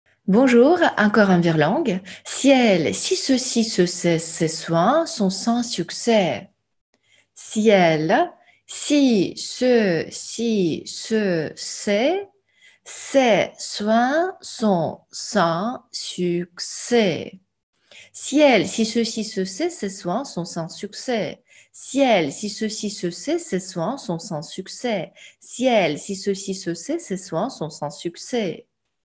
Скороговорка на французском №21: Ciel, si ceci se sait ces soins sont sans succès
👌🏻Тренировать произношение с нами — легко! А еще это прекрасная возможность посмеяться, ведь с первого раза произнести скороговорку правильно практически невозможно)

Ciel, si ceci se sait ces soins sont sans succès.

Читаем сначала медленно, потом по слогам, потом быстро 3 раза.

Попробуем перевести с вами?)